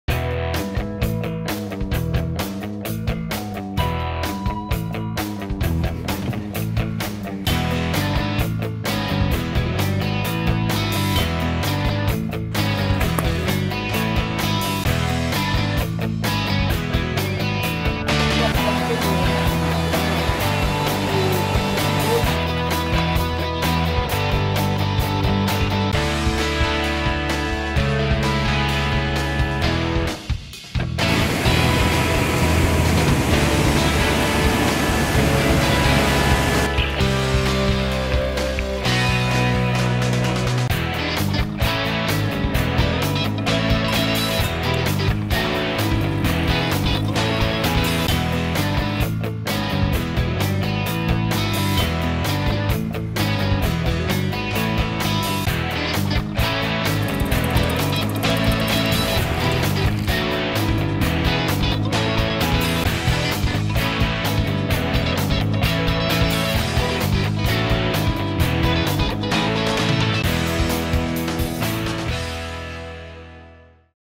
E-Mountainbike Tour in Paneveggio Park (Italian Alps) with DailyGreenspiration.com
Making a cycling tour through a hilly forest is great using an electric mountain bike, really, it is cool!